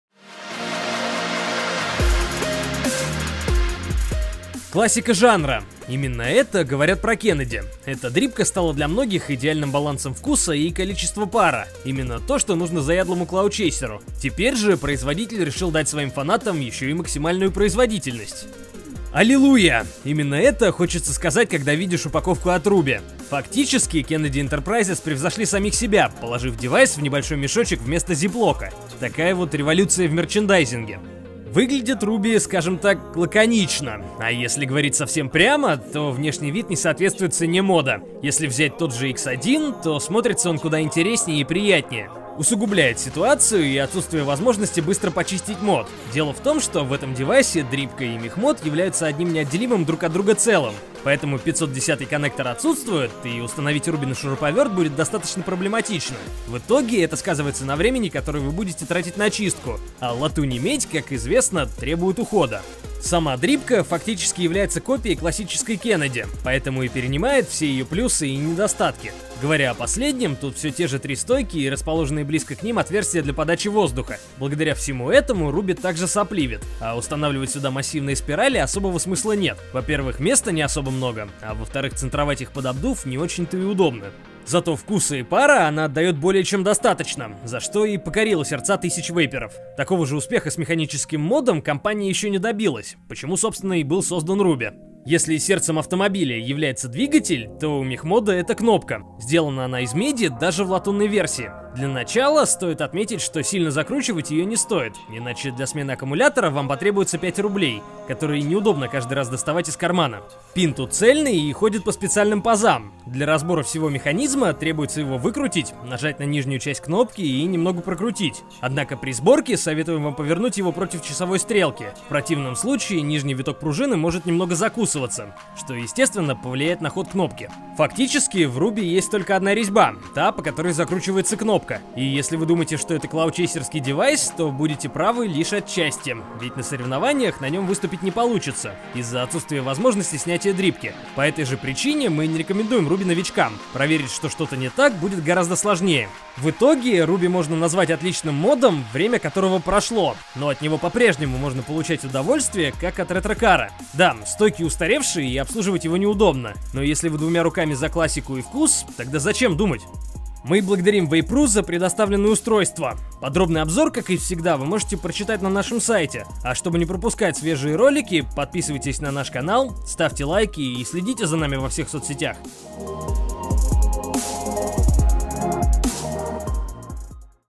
Ruby Mod от Kennedy Enterprises | Обзор
Дрипка Kennedy уже давно стала классикой и эталоном баланса вкуса и количества пара. Что нужно заядлому клаудчейсеру на каждый день? Минимум просадок, максимум пара и столько же вкуса. Именно это и предлагает Ruby mod. Но что же он представляет из себя на самом деле?

Спасибо Vape.ru за предоставленный на обзор девайс.